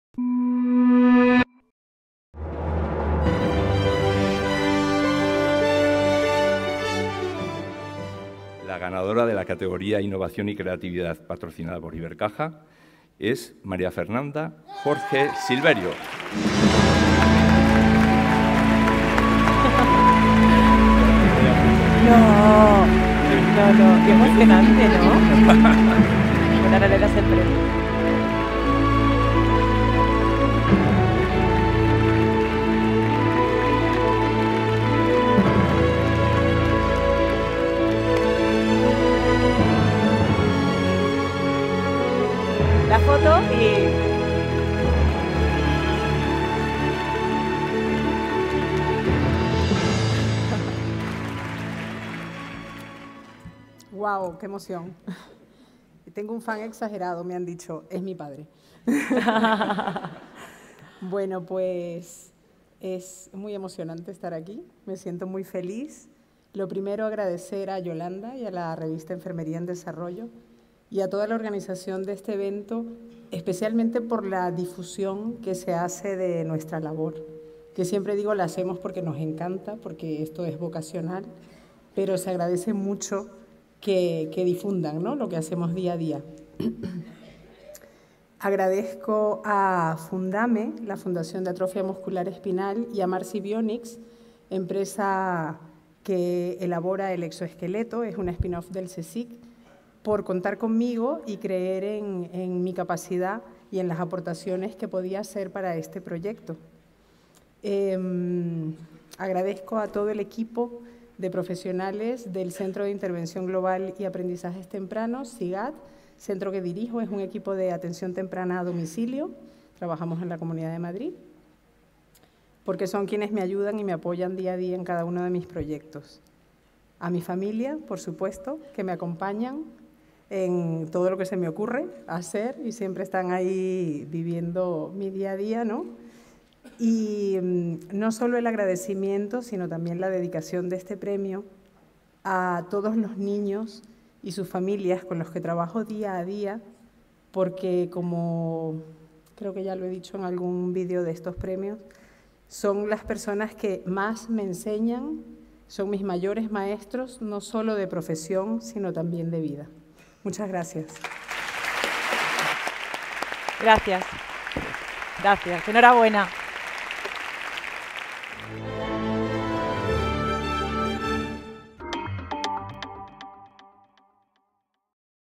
Discurso de la ganadora PremiosED Innovación
La fisioterapeuta Mª Fernanda Jorge es la ganadora de PremiosED en la categoría de Innovación. Se lo entregó Fernando Arcega.